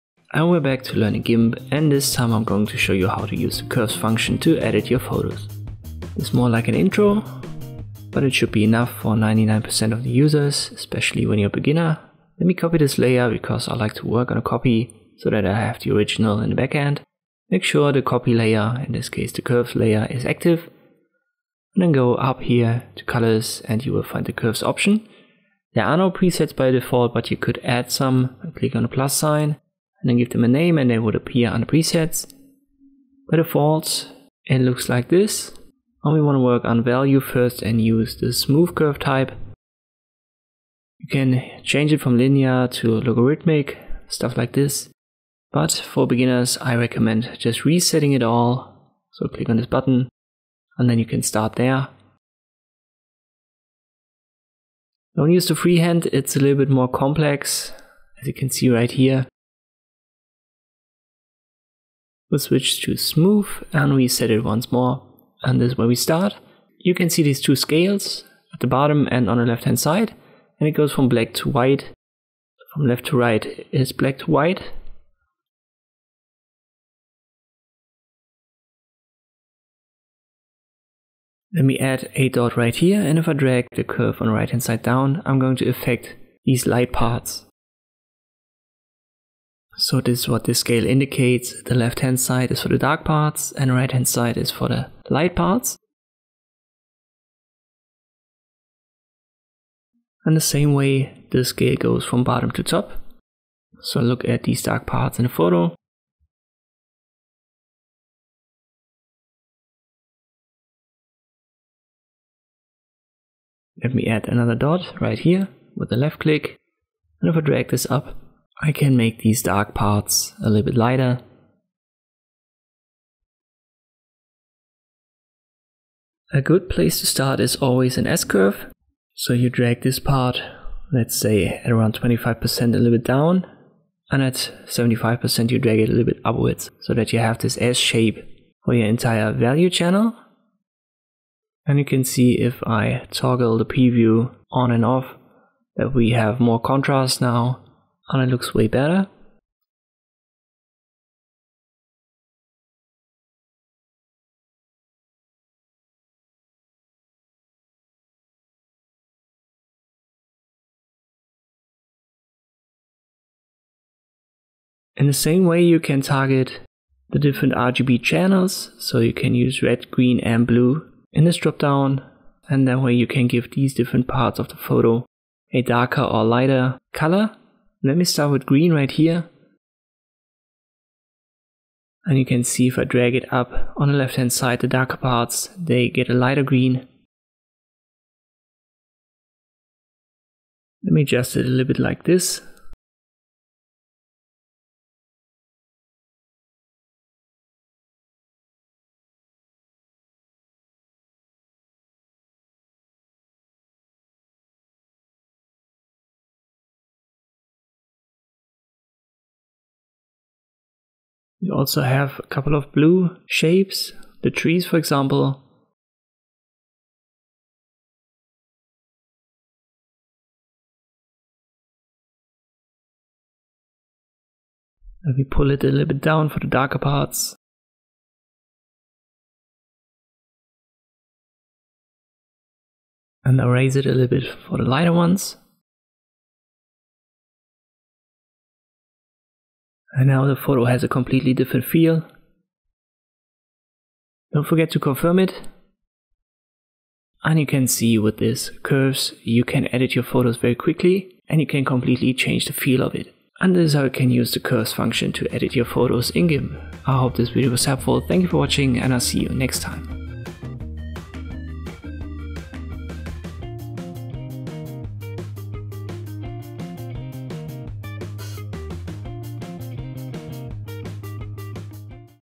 How to Use the Curves Function to Edit Photos in Gimp
In this tutorial I'll show you how to use the curves function to edit photos in Gimp.